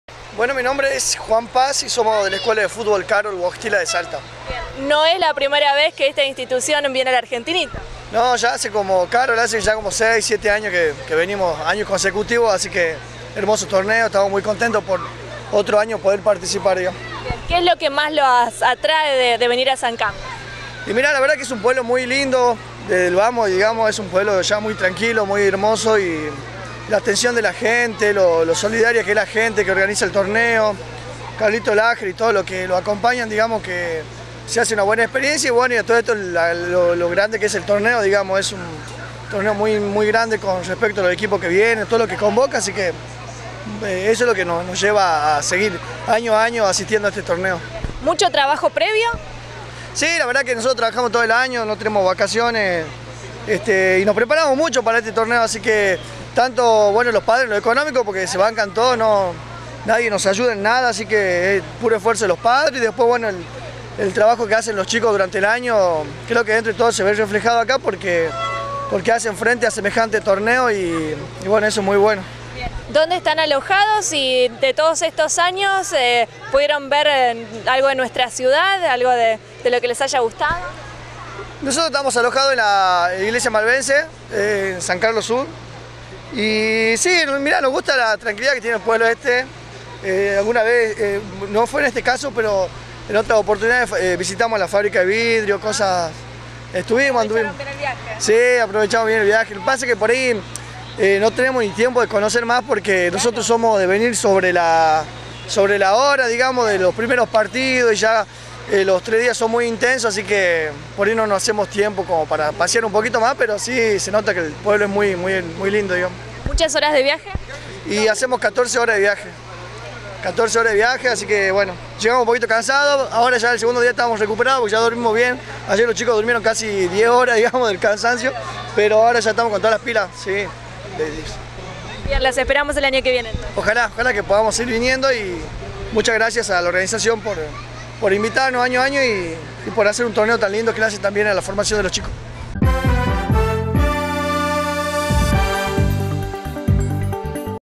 Club Escuela De futbol de Salta
El Argentinito tuvo otra edición exitosa